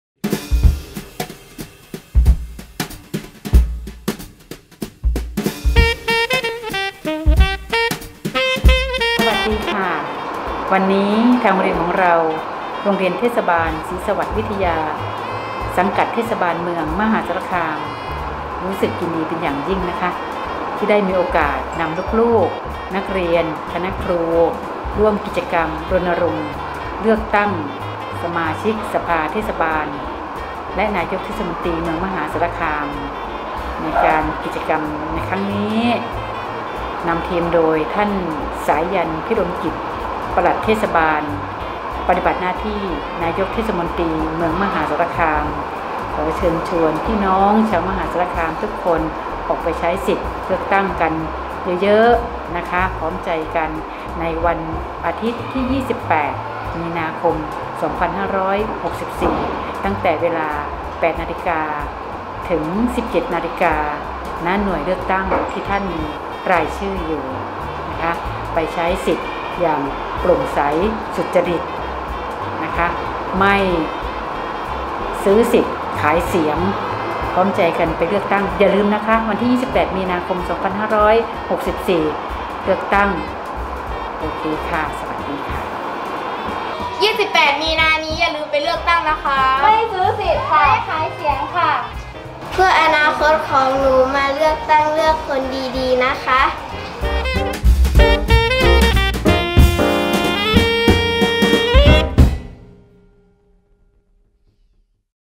ผู้อำนวยการโรงเรียนเทศบาลศรีสวัสดิ์วิทยา ขอเชิญชวนเลืกอตั้ง
ผู้อำนวยการโรงเรียนเทศบาลศรีสวัสดิ์วิทยา ขอเชิญชวนเลืกอตั้งสมาชิกสภาเทศบาลและนายกเทศมนตรีเมืองมหาสารคาม